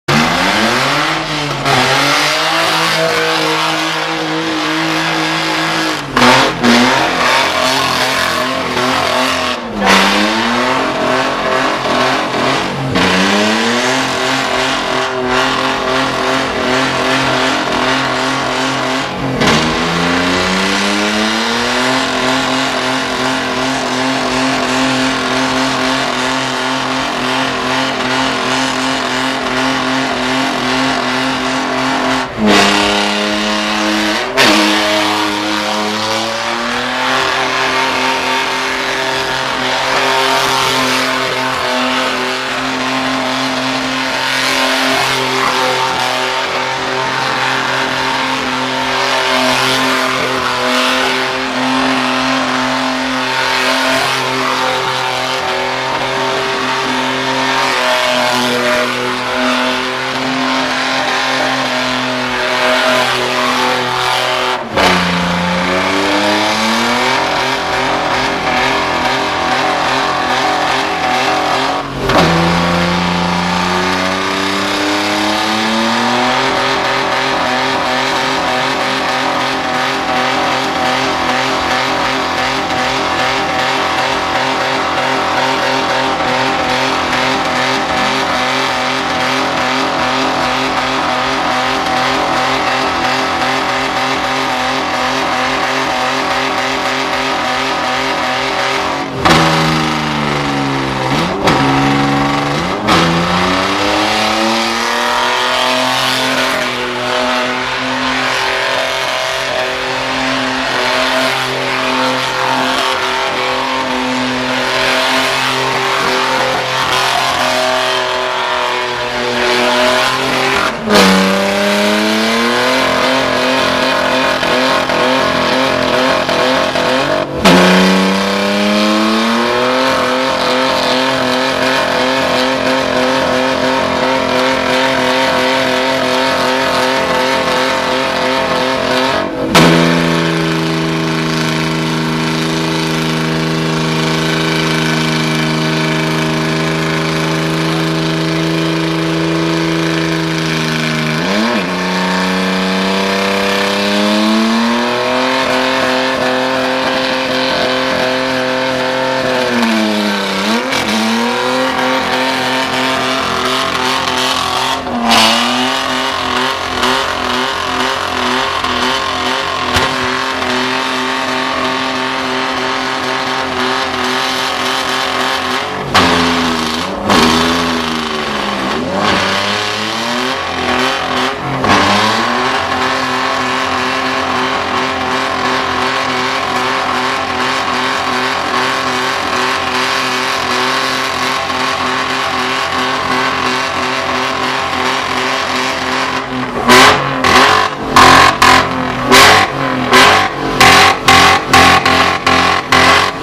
Mistrz pokazu mocy klasa PRO - BMW E30 328 "KOCIAK" - Majówka z BMW Toruń 2018
Zwycięzca klasy PRO. Który to już tytuł ? Czwarty chyba :)